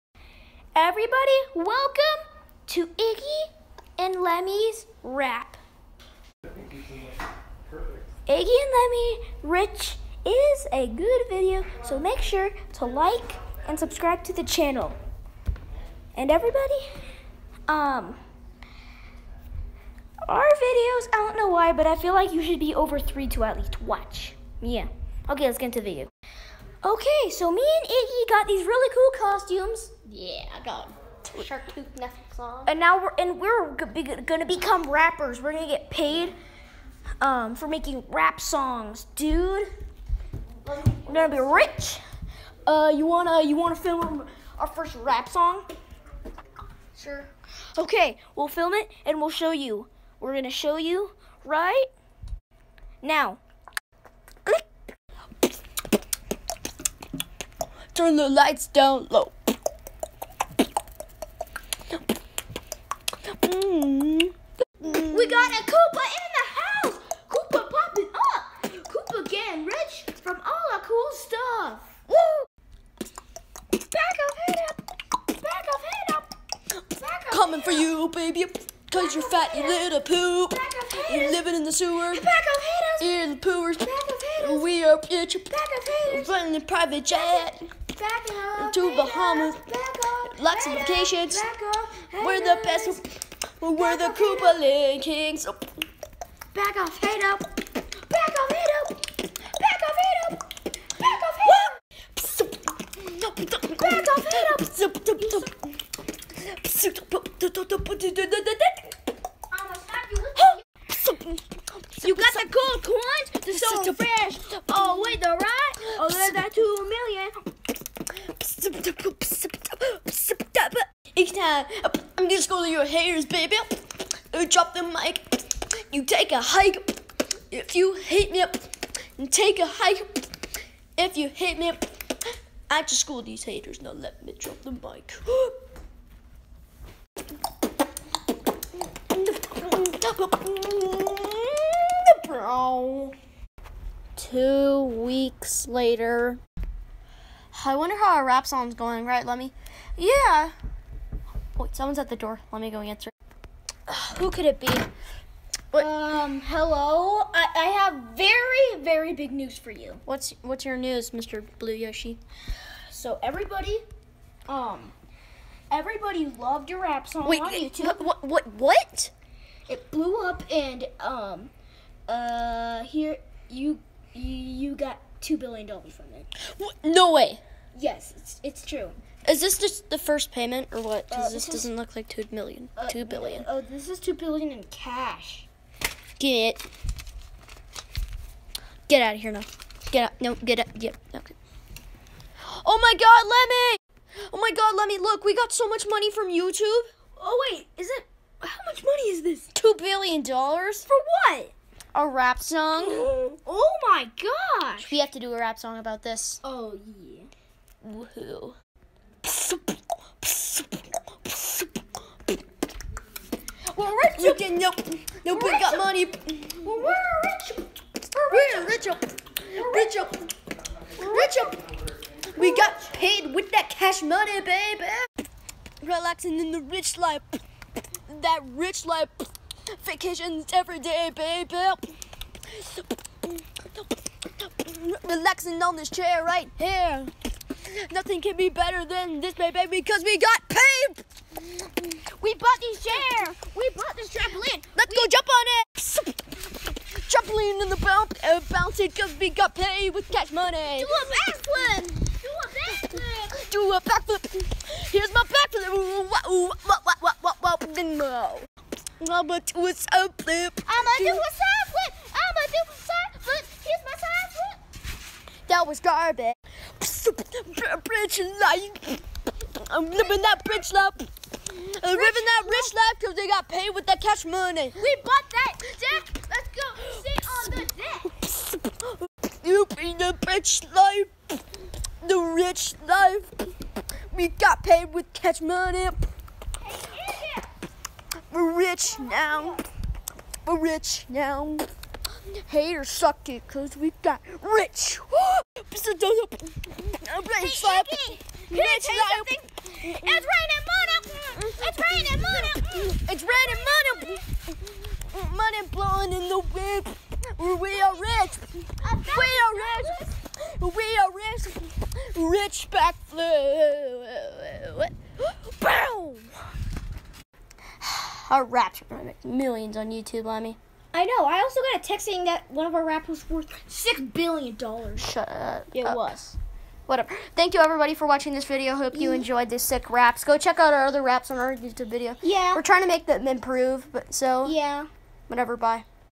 Lemmys rich
Lemmy and Iggy do raps and become rich what do they do?